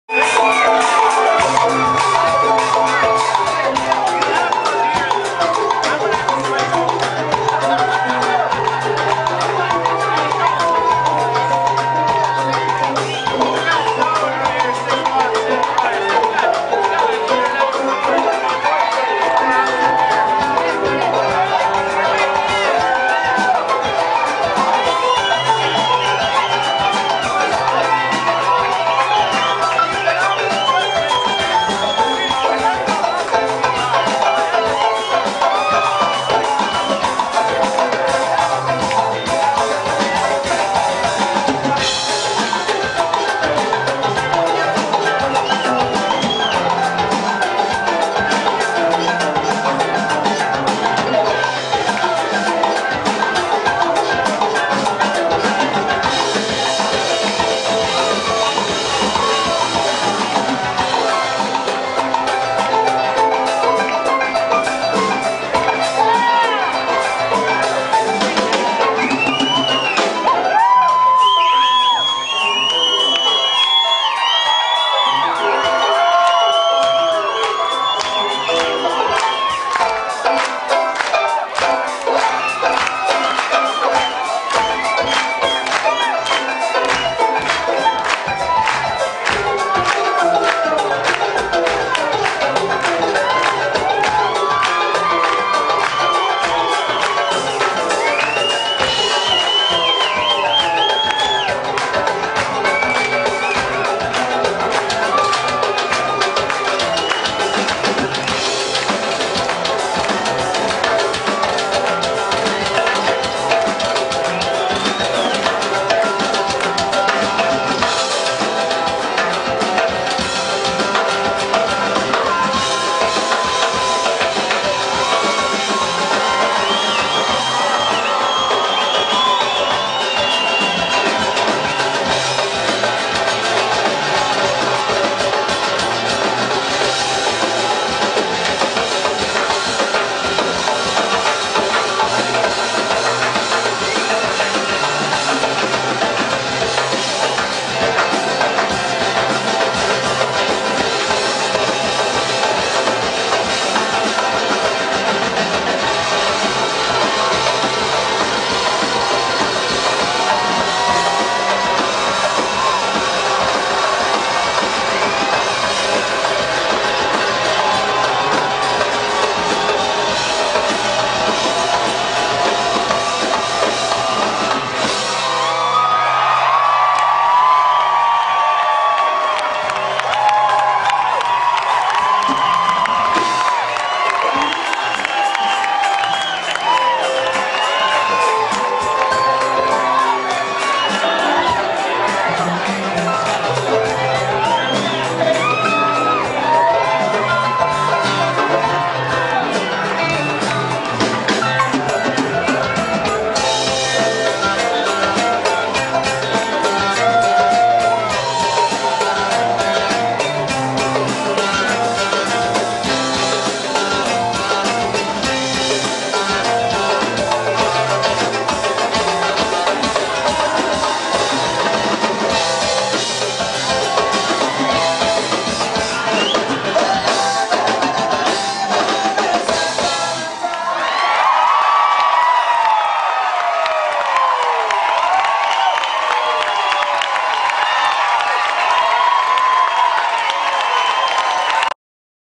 Ozomatli Live - jamming with Xoco - The Harpist from Rey Fresco
During Ozomatlis recent headline set at the Belly Up tavern in Solana Beach, CA, they invited Rey Frescos harp player, Xoco, to join them on stage. What took place was rousing instrumental jam that had the audience on their feet as Xoco raged on a massive harp alongside the band.